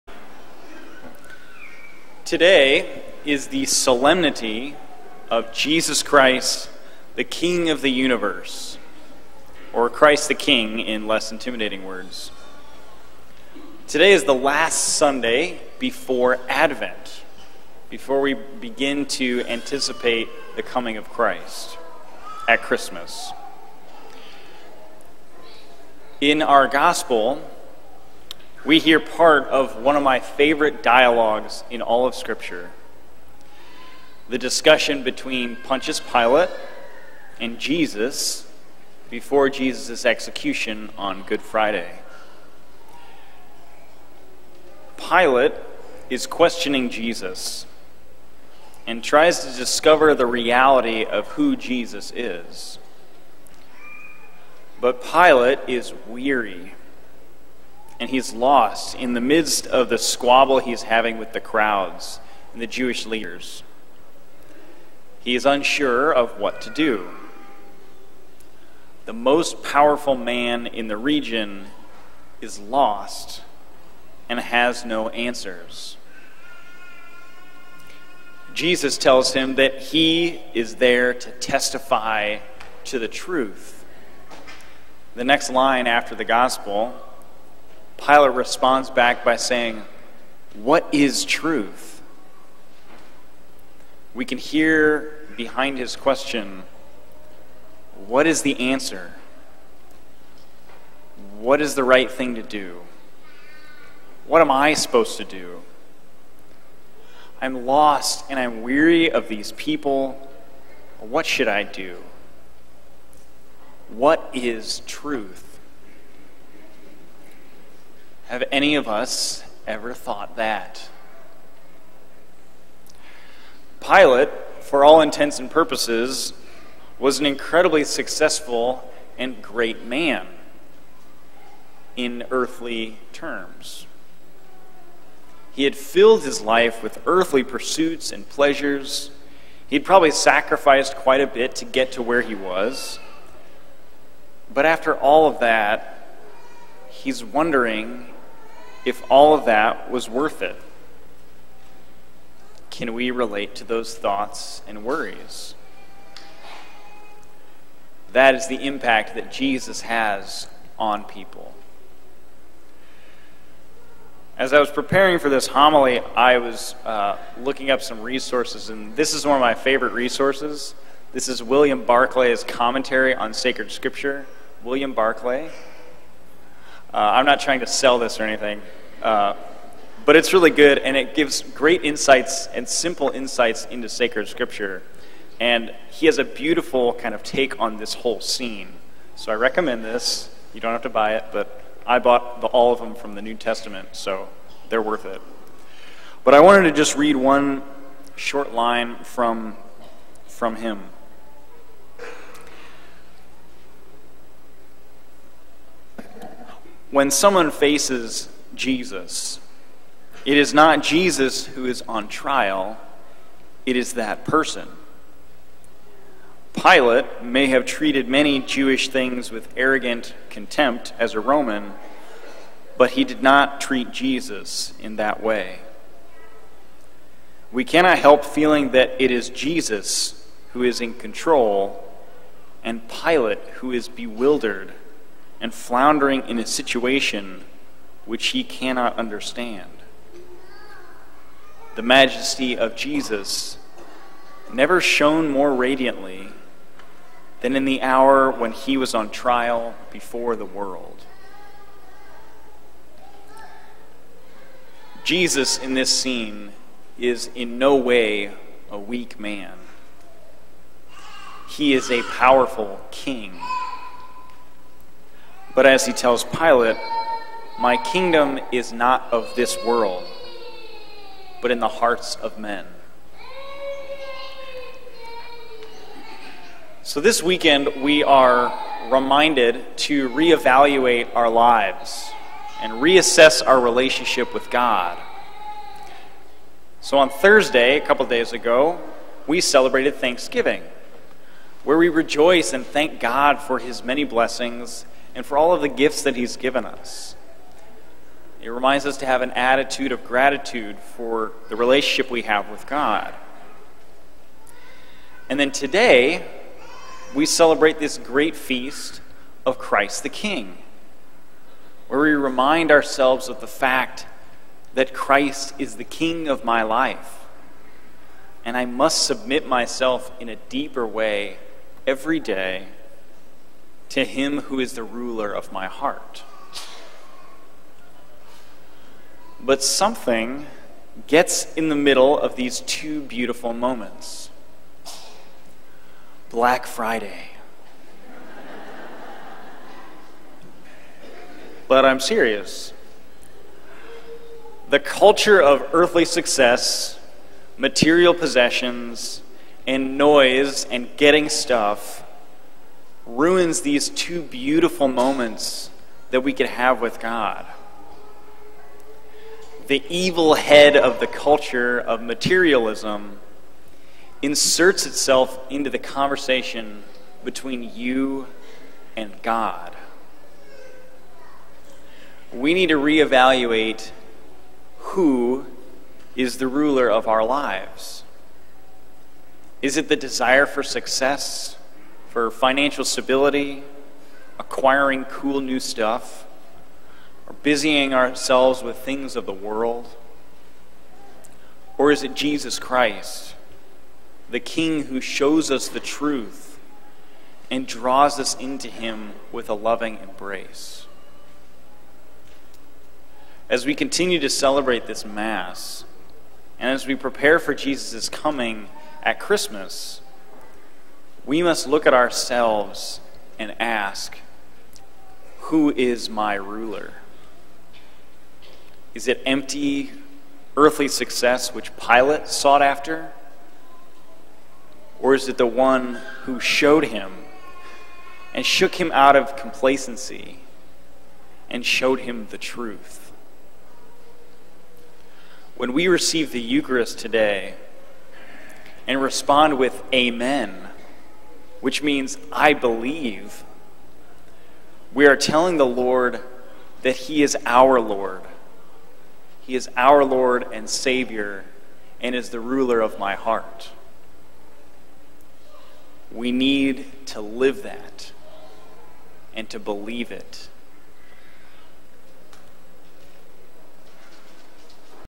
Homily: Our Lord Jesus Christ, King of the Universe
Homily by Fr. Thomas Zehr

11/25/18